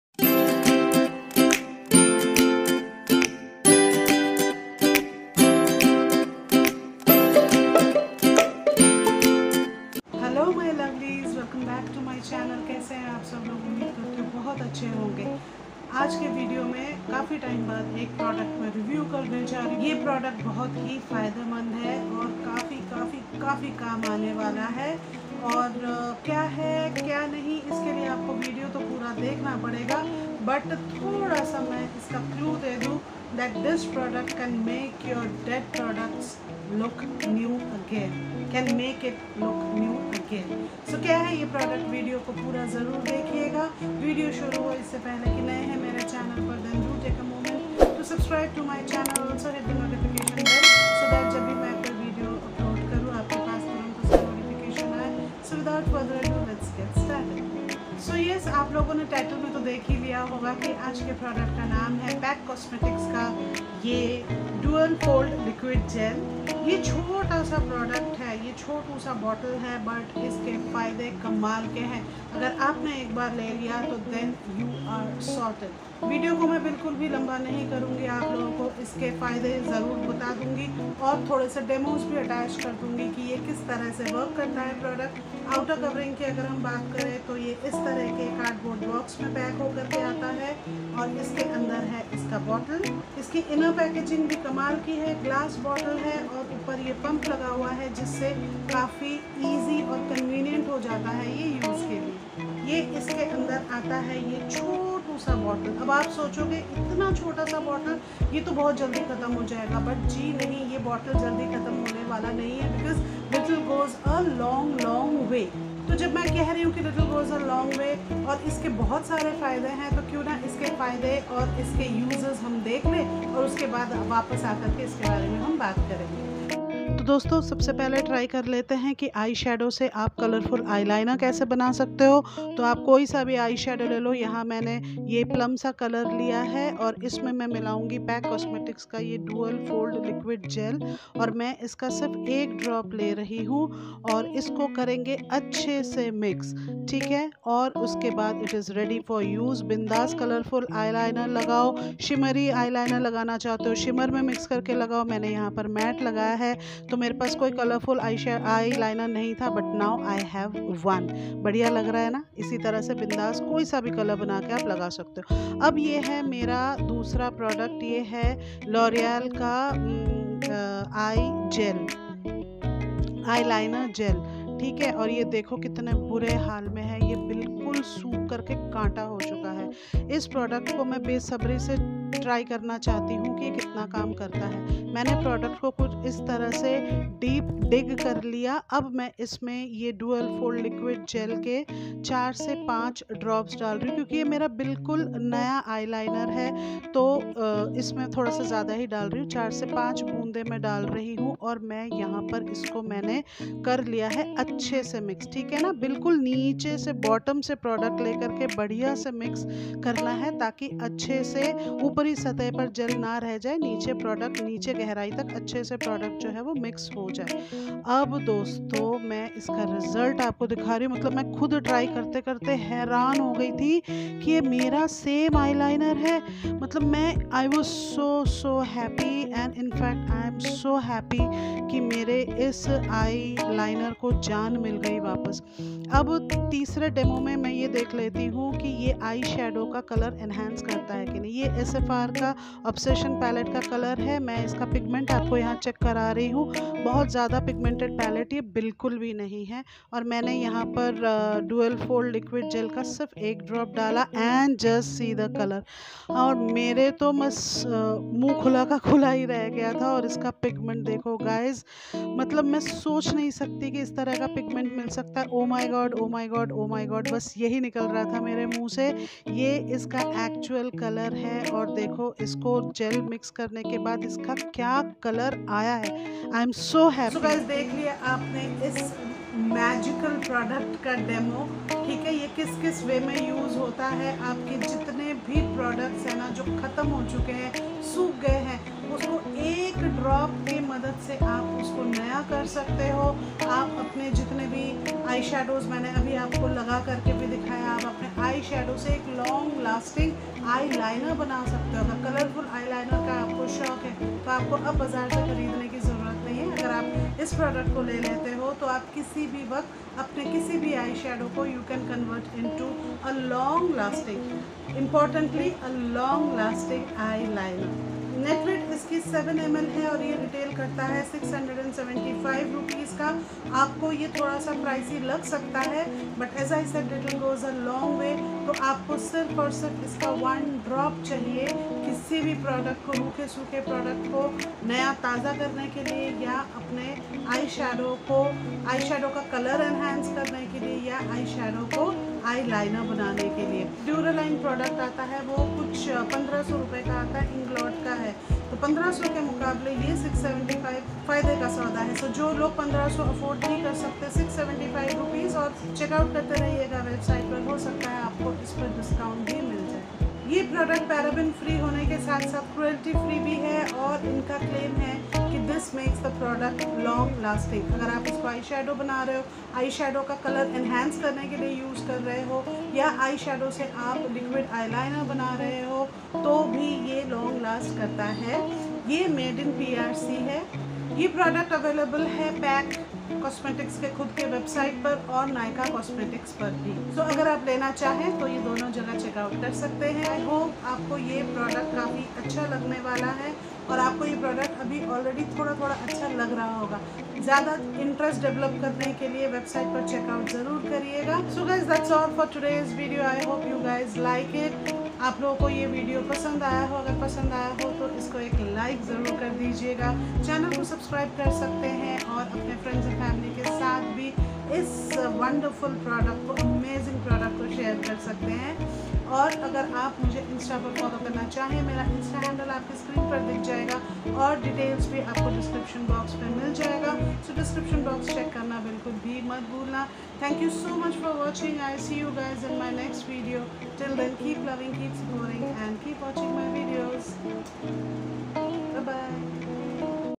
PAC Dual-Fold Liquid Gel Review | How to Fix Dry Gel Liner| Shringar Forever
Today im reviewing a very interesting product which is PAC Cosmetics Dual Fold Liquid Gel. I loved filming this product & i hope you guys will love watching it.

Videos you May like to watch-